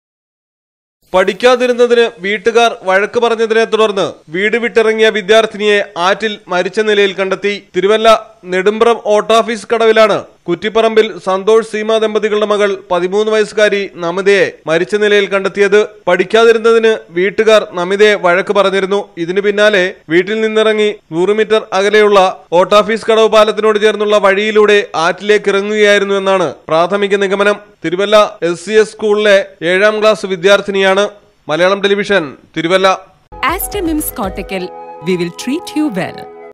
വീട്ടുകാർ വഴക്കു പറഞ്ഞതിനു വീടുവിട്ടിറങ്ങിയ പെൺകുട്ടി ആറ്റിൽ മരിച്ച നിലയിൽ
ഏഴാം ക്ലാസുകാരി ആറ്റിൽ മരിച്ച നിലയിൽ, വീട്ടുകാർ വഴക്ക് പറഞ്ഞതിനെ തുടർന്ന് വീട് വിട്ട് ഇറങ്ങിയതായിരുന്നു കുട്ടി.